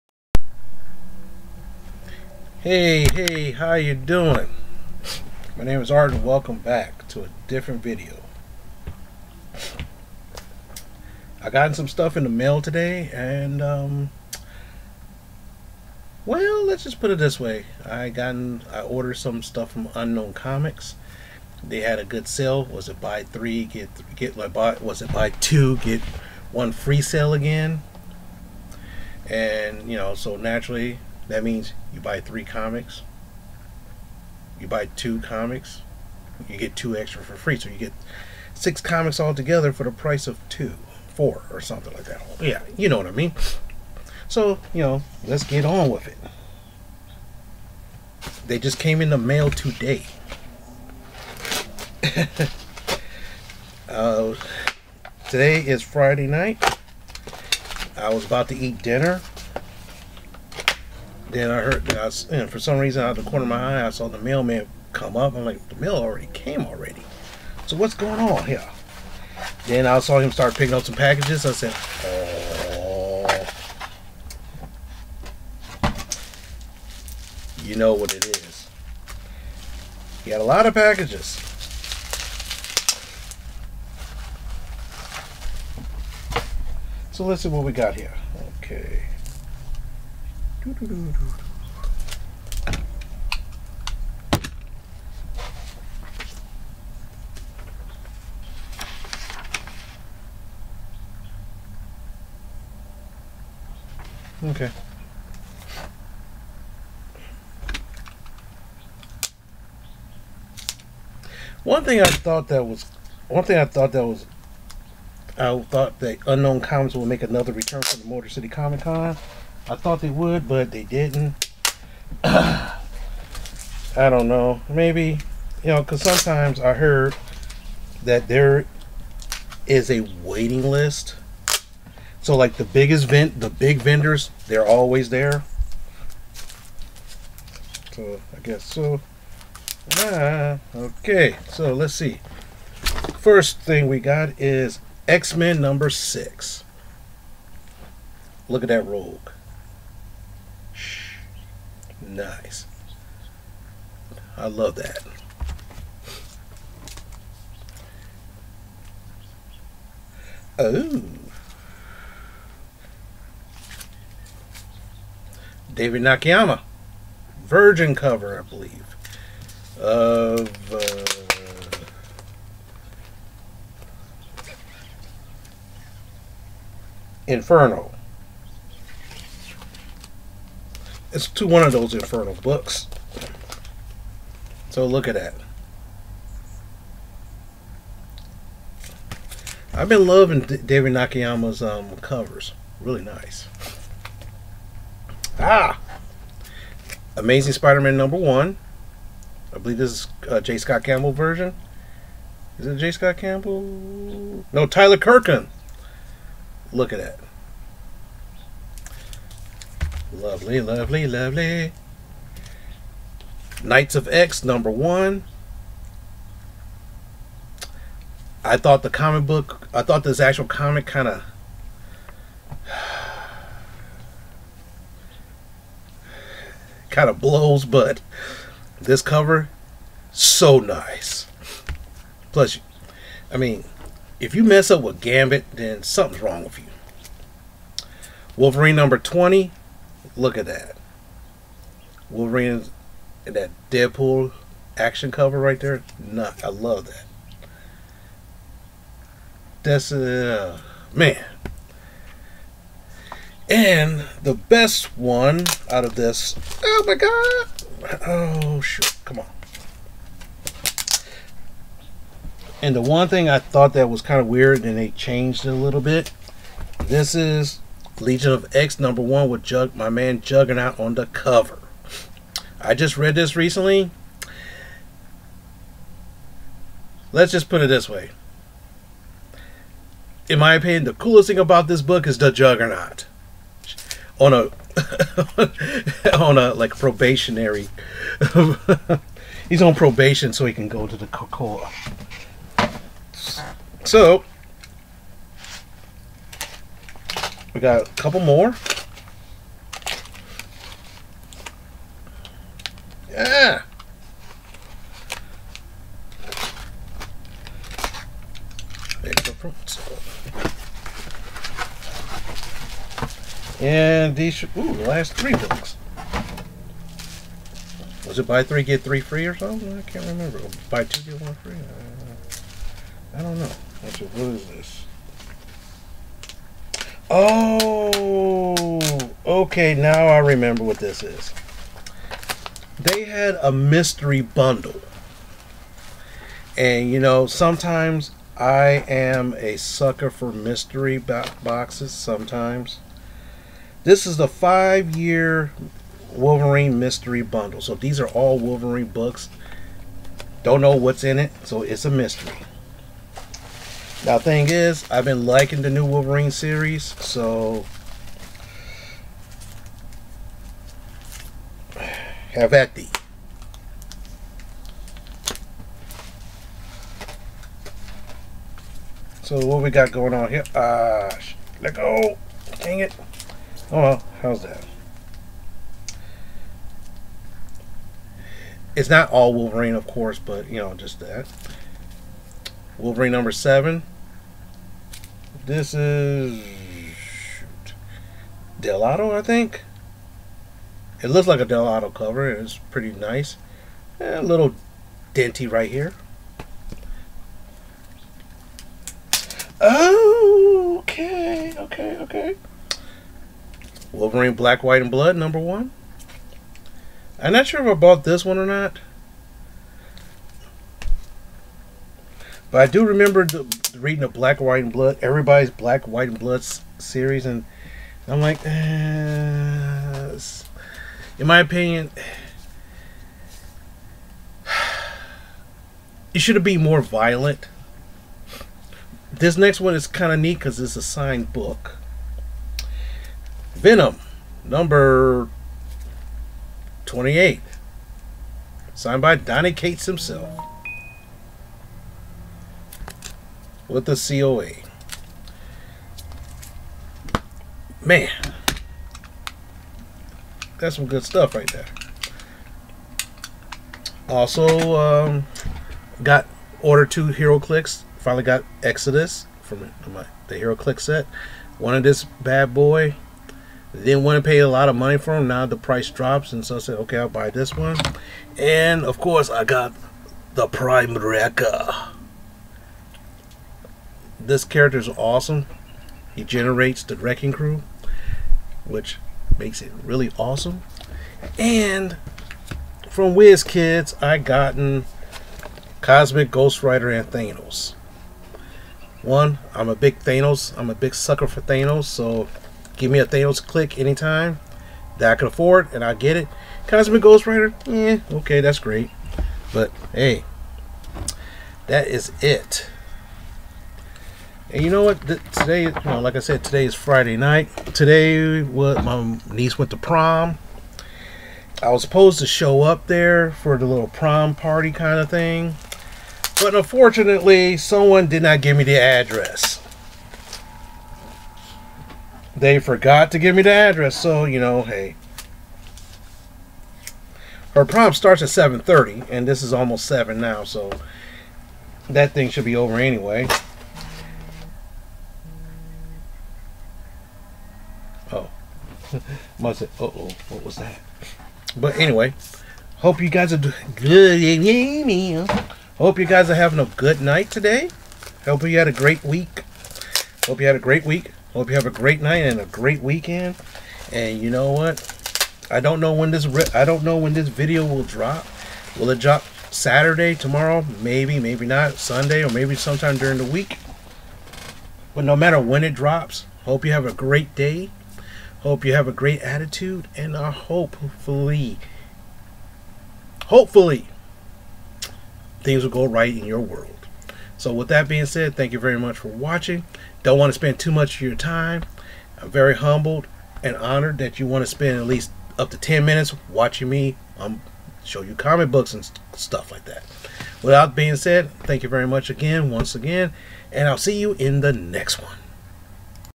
Unknown comics book haul and minor stuff
I bought some books from Unknown Comics during a sale and here it is